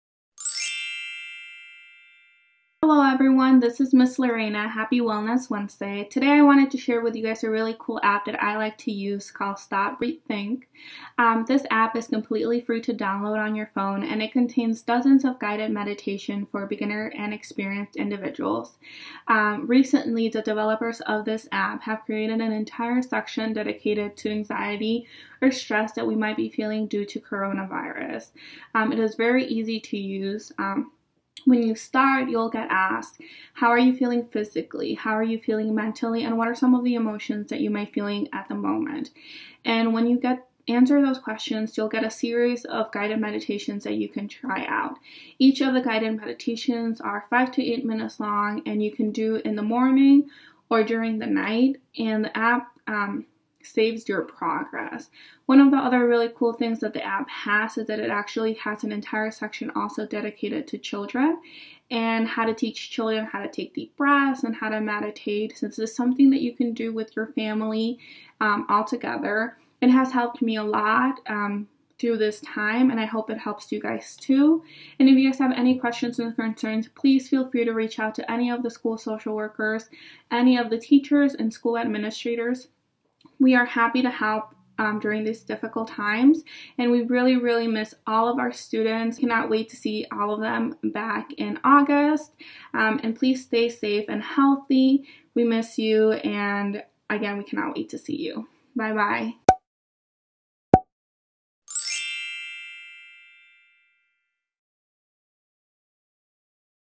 Wellness Wednesday - Stop. Breathe. Think.
Crosby Elementary's Lorena Villagomez shares the guided meditation app The app offers programs for both adults and children, and it's available for both Android and iOS devices. It's a terrific tool to help cope with stress and uncertainty.